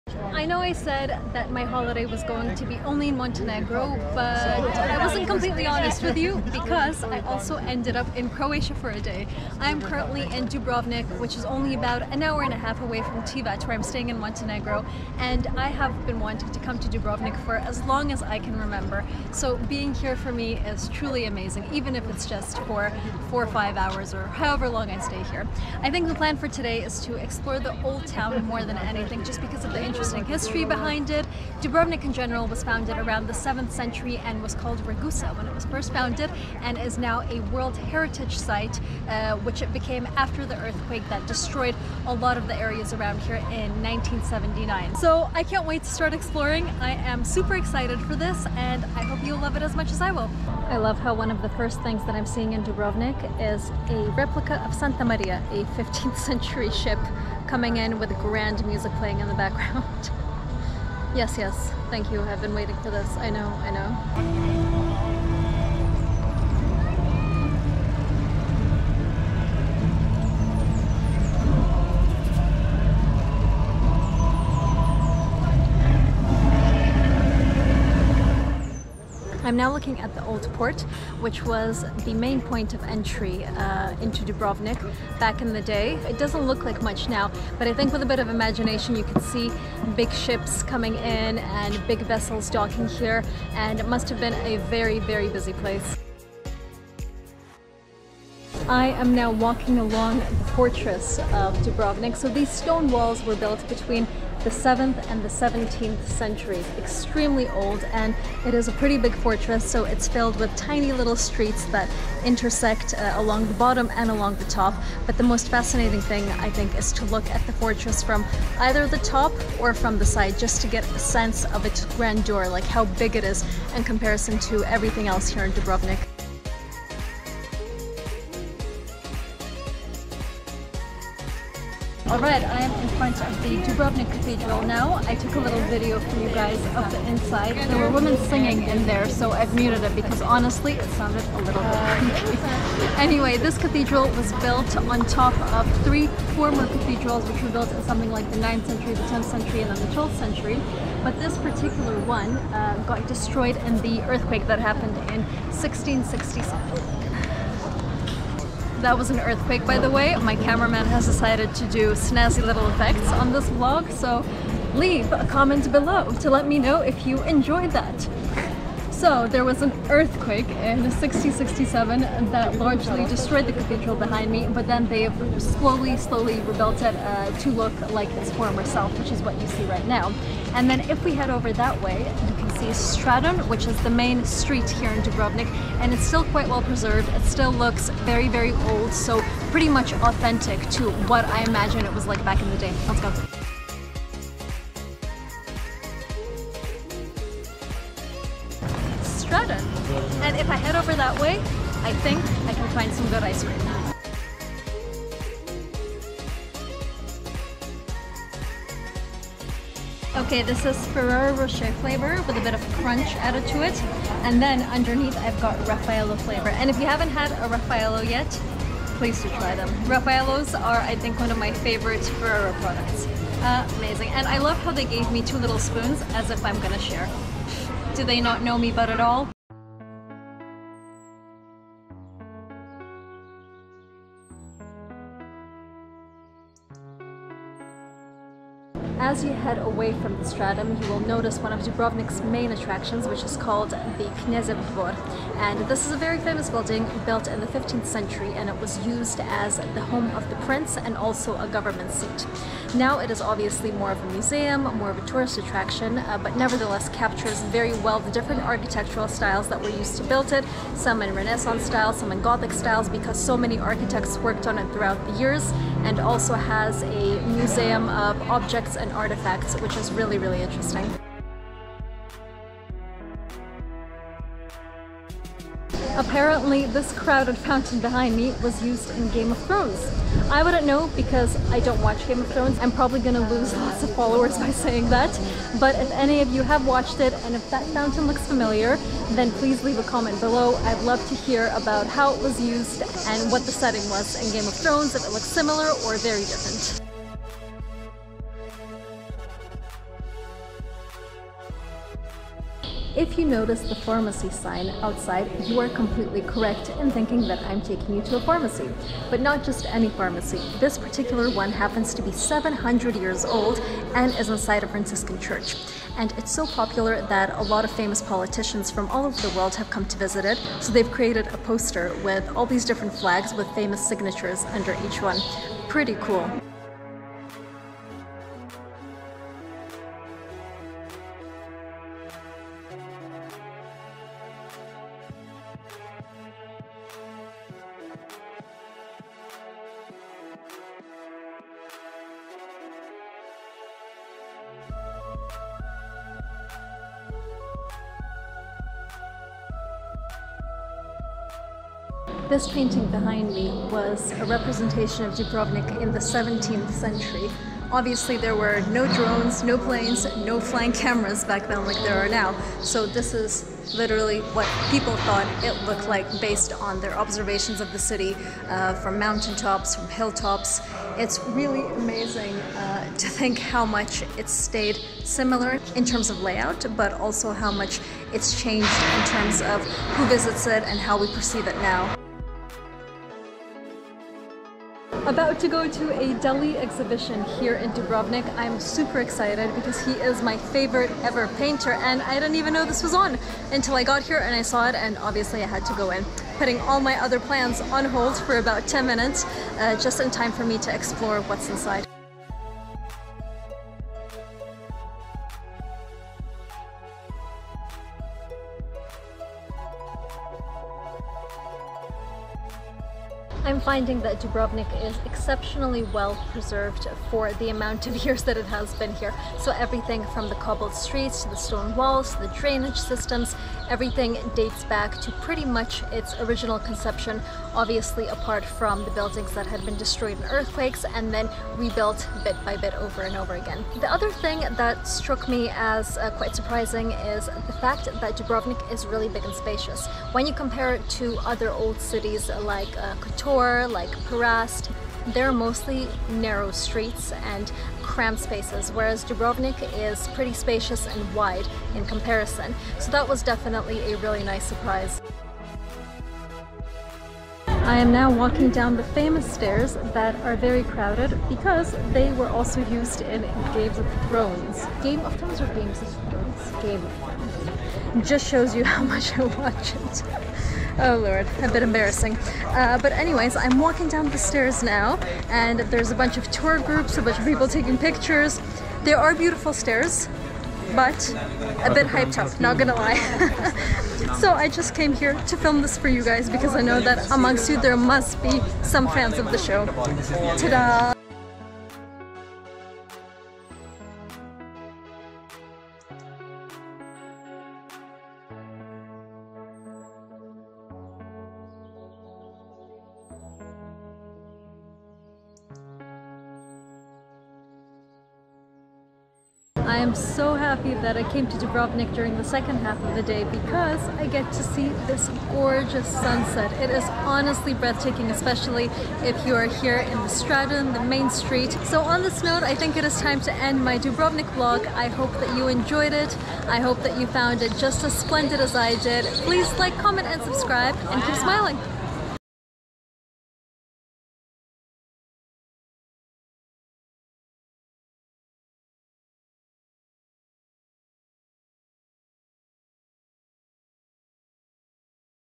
DUBROVNIK Walking Tour! // ONE DAY GUIDE !
I take you on a Dubrovnik walking tour during my first time in the city. We see historical landmarks, old streets in the fortress, famous filming scenes used in Game of Thrones, a 700 year old pharmacy, Dubrovnik's best ice cream place and an unexpected art exhibition from my favourite painter! The vlog finishes with a breath-taking sunset. Coming here was always a dream and I'm so happy I got to share my experience with you. Watch this quick tripdetailing how you can see Dubrovnik in one day in 4K for best quality.

Get my monthly guides, top travel hacks and freebies sent to your email

TIME STAMPS:

0:00 INTRO
0:50 Santa Maria ship replica and Dubrovnik port
1:35 Dubrovnik Fortress
2:12 Dubrovnik Cathedral and Stradun
3:20 Peppino's Ice Cream !
3:49 Knezev Dvor Dubrovnik
4:31 GOT fountain Dubrovnik
4:56 Old Pharmacy Dubrovnik
6:22 Dali expo Dubrovnik
6:42 Dubrovnik streets
7:37 Jesuit Stairs Dubrovnik
8:29 OUTTRO